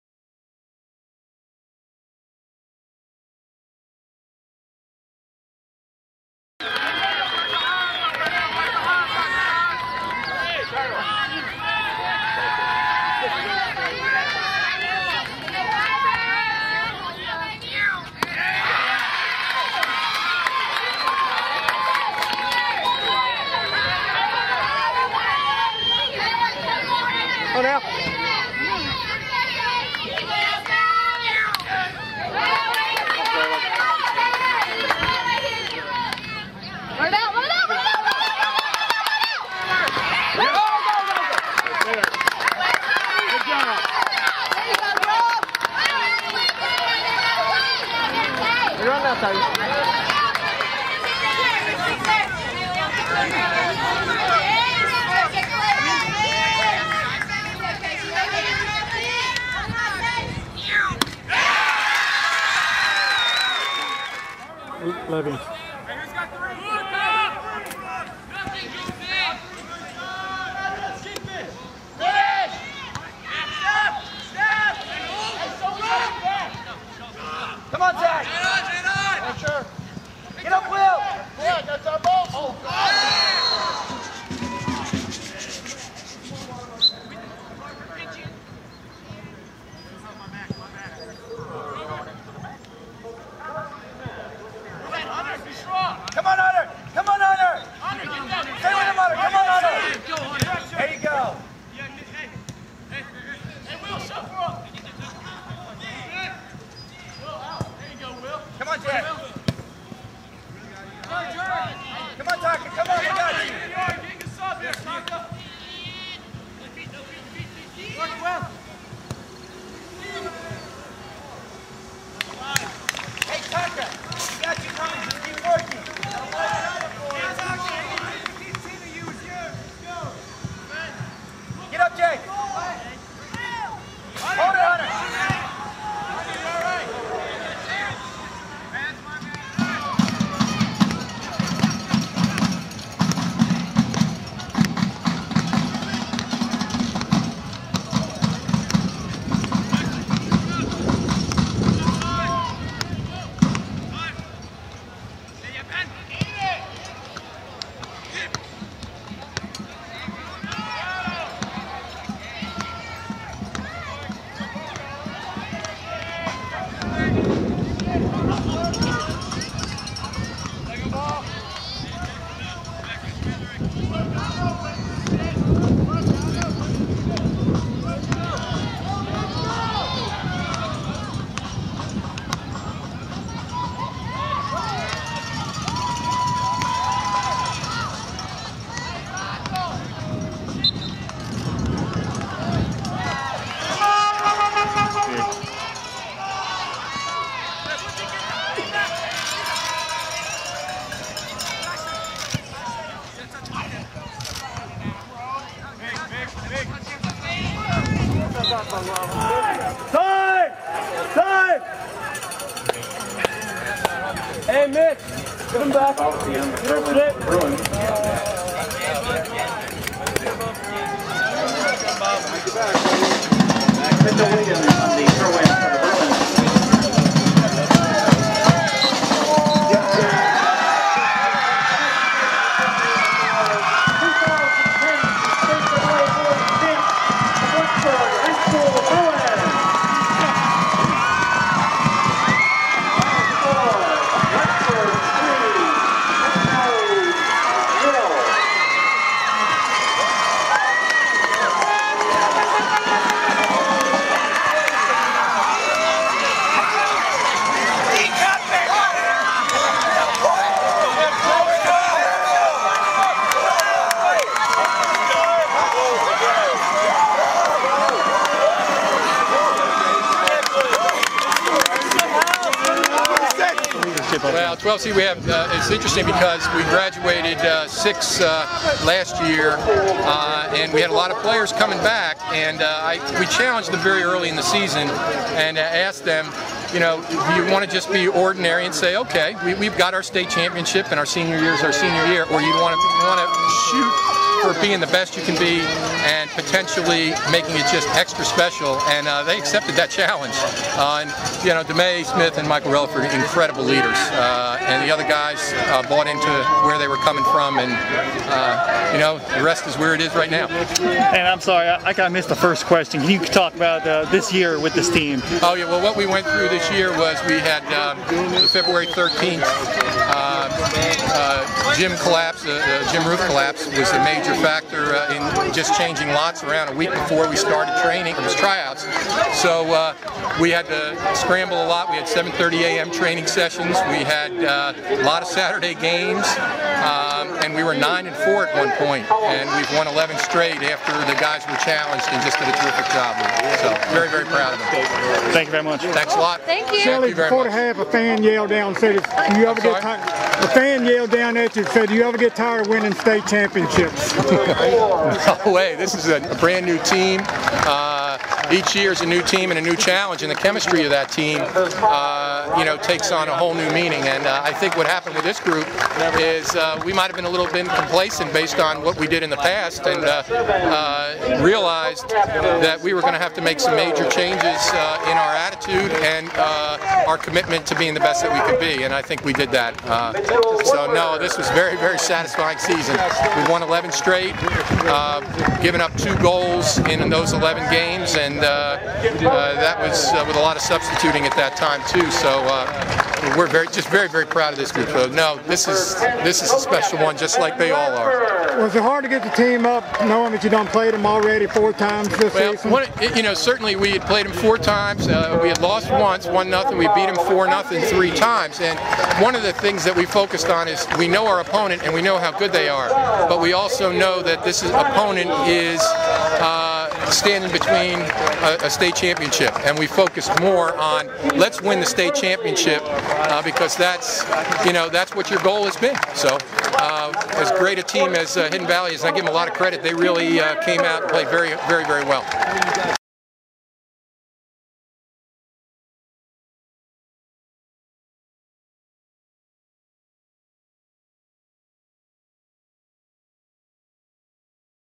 Group A_AA State Championships 6_13_10
Highlites from the Group A/AA state championship games played on Sunday, June 13, 2010 at Radford University. Video features the Abingdon/Northside championship softball game, the George Mason/Radford boys soccer championship match and the Hidden Valley/Blacksburg boys soccer championship match.